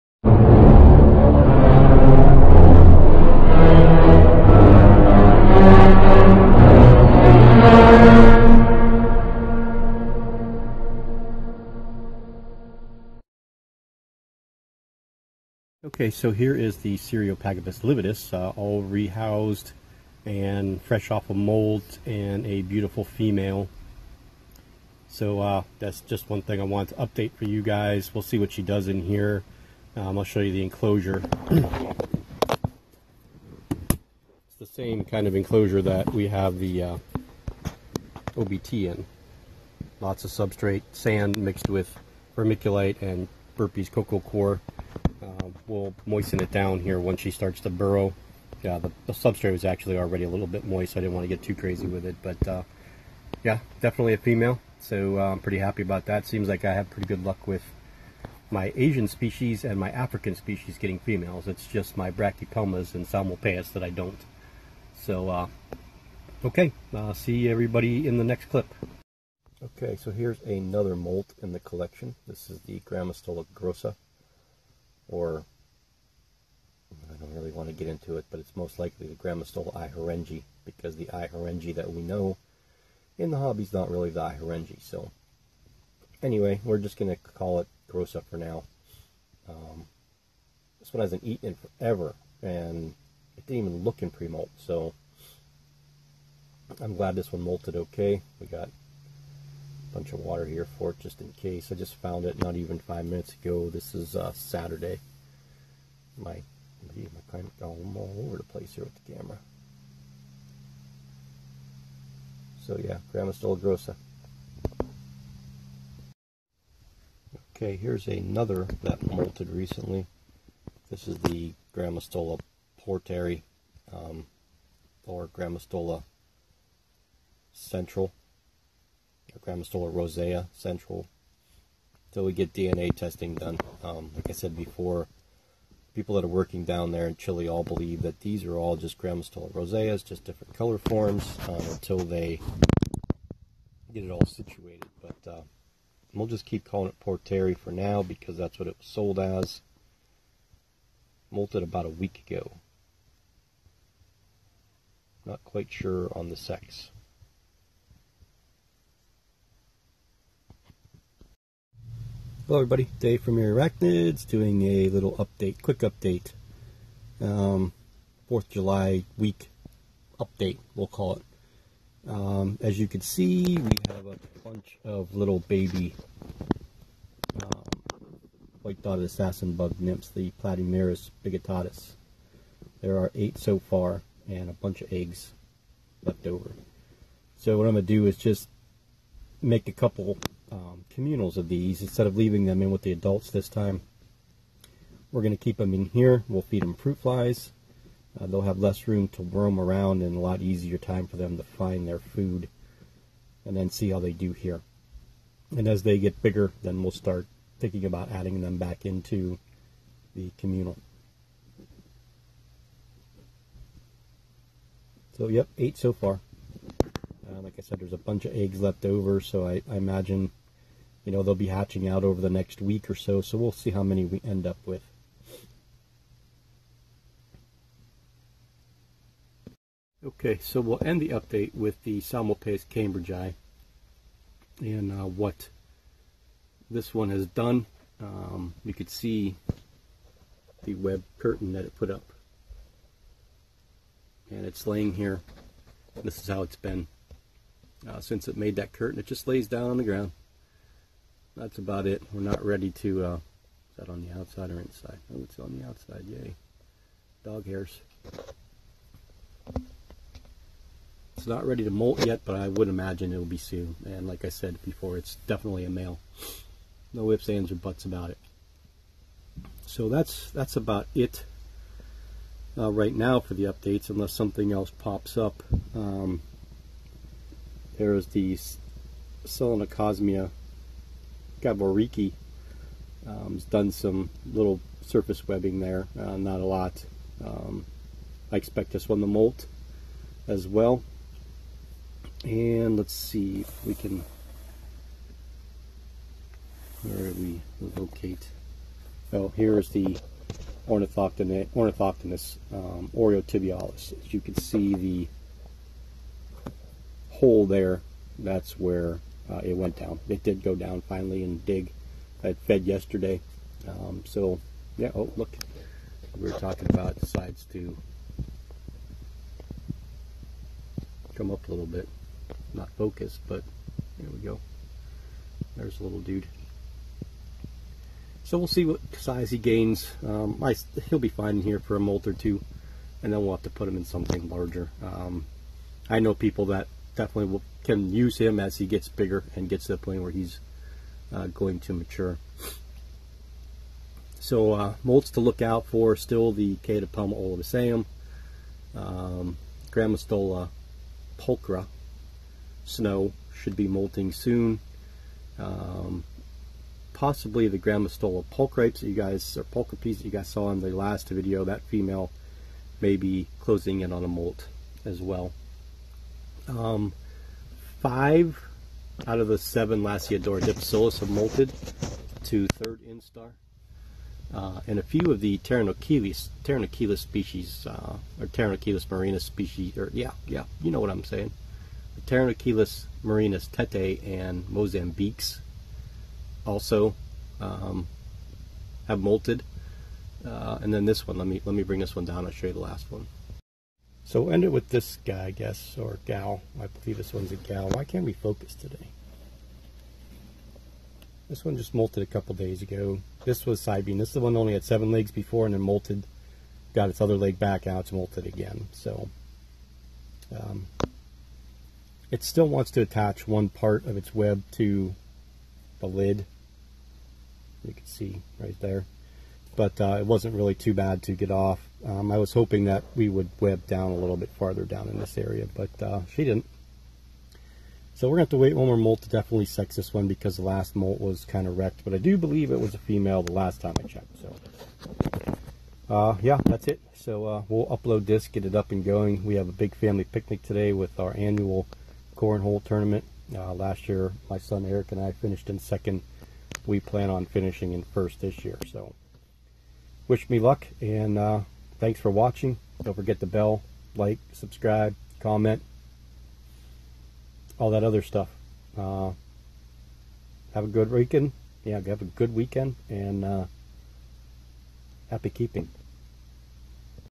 Assassin Bug Nymphs started to hatch and some Molt Updates
A quick video to update you one some tarantulas that molted and the new baby assassin bugs.
Platymerus biguttatus..African White Dotted Assassin Bugs